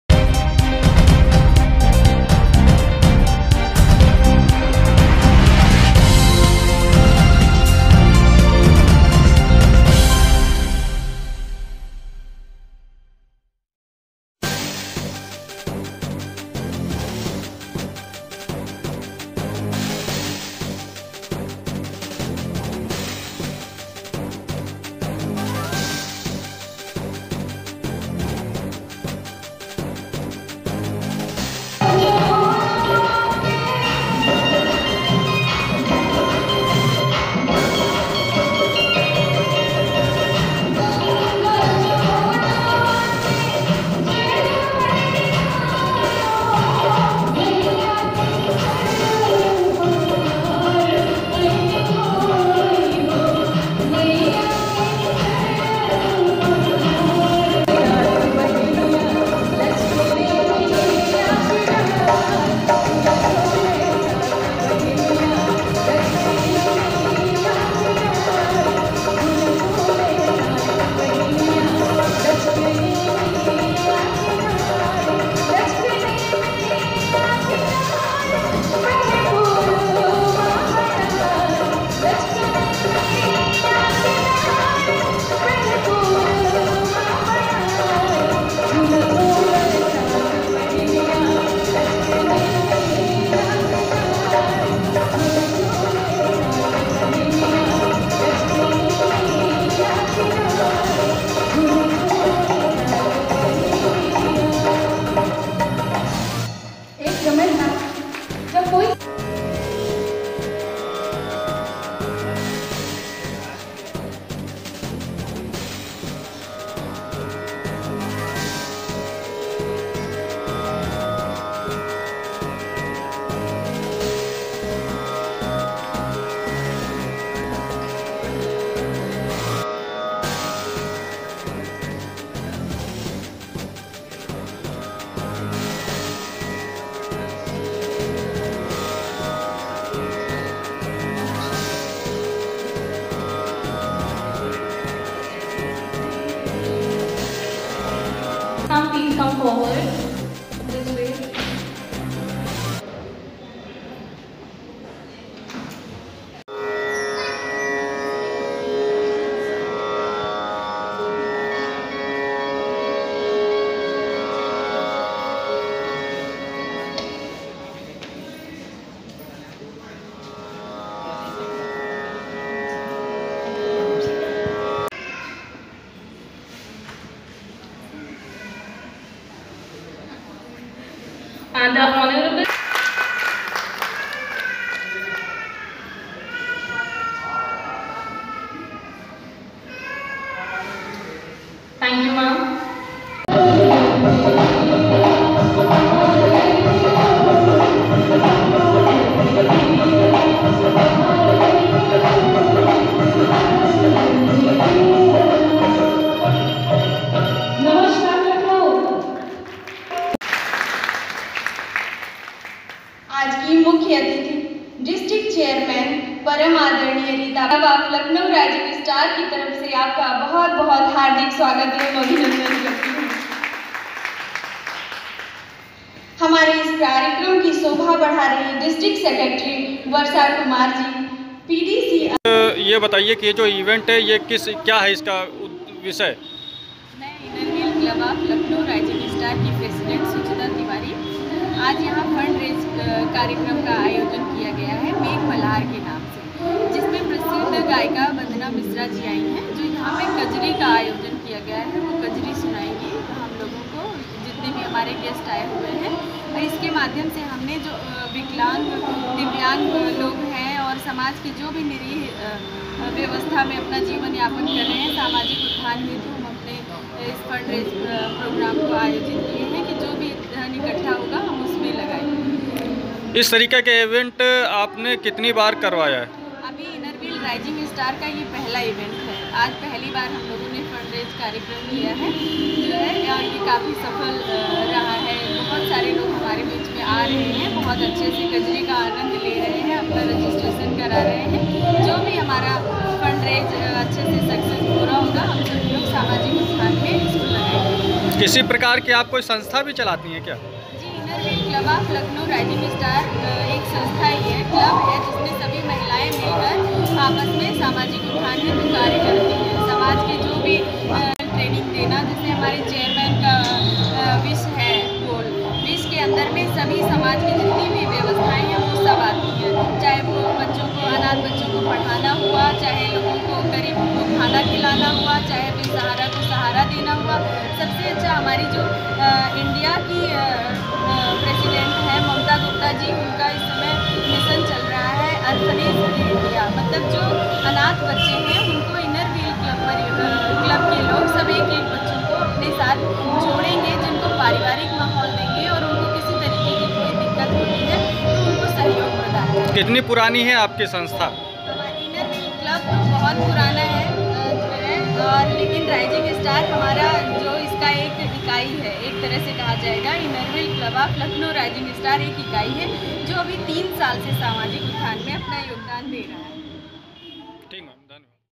लखनऊ के सीएमएस स्कूल में इनरव्हील क्लब की अध्यक्ष सुचिता तिवारी द्वारा मेघ मल्हार कार्यक्रम का आयोजन
लखनऊ के सीएमएस स्कूल में इनरव्हील क्लब की अध्यक्ष सुचिता तिवारी द्वारा मेघ मल्हार कार्यक्रम का आयोजन किया गया